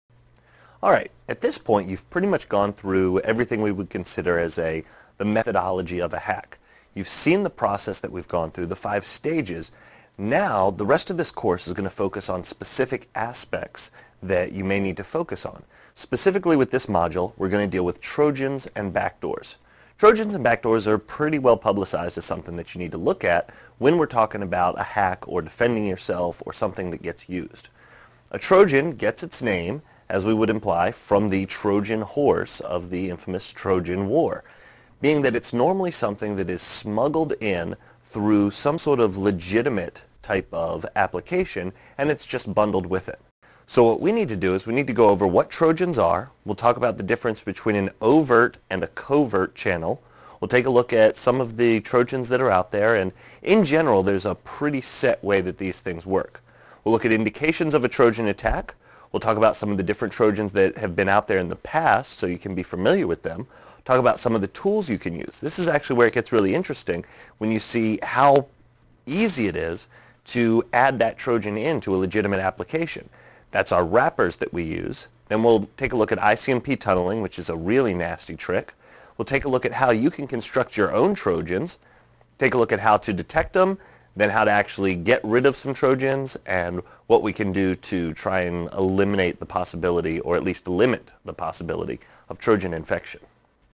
CEH v6 Module 8 from QuickCert: Trojans and Backdoors
System hacking demo from QuickCert's EC-Council CEH training course.  QuickCert's Certified Ethical Hacker or CEH is a vendor-neutral certification that explores the existence and the use of hacking tools and security systems across 22 topical domains.